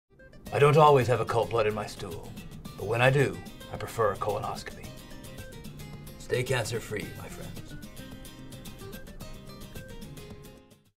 Stay thirsty, my friends
What does the most interesting man in the world do when he has occult blood in his stool?